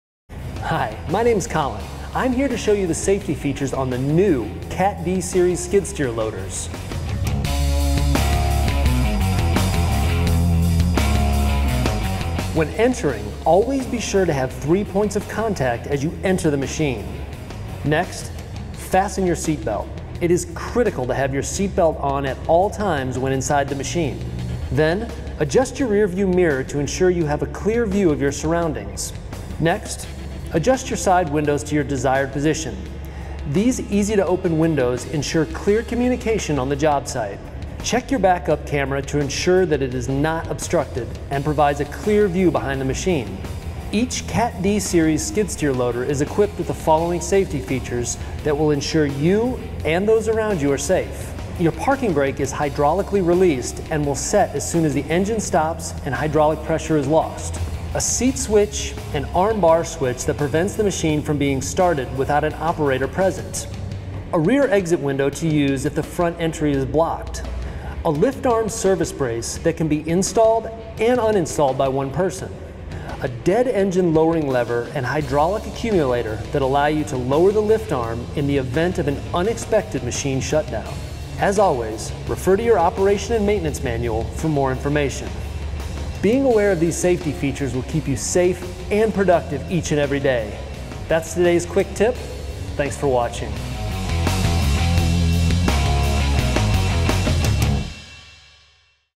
Quick Tips: Cat® Equipment Safety
At MacAllister Machinery, we take safety seriously and want to share this video as a reminder of the basic steps you can take to ensure the safety of yourself, your coworkers and the people around you.

Learn more about the safety features on the new Cat® D Series Skid Steer Loaders